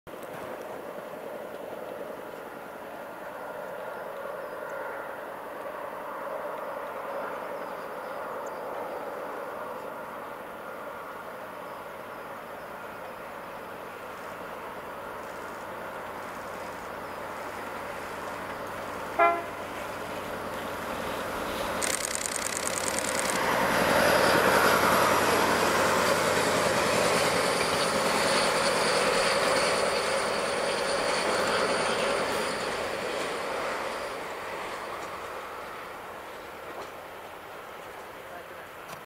宗谷線ラッセル DE15 その1
宗谷線のラッセル、DE15です。残念ながら、ウィングは閉じています。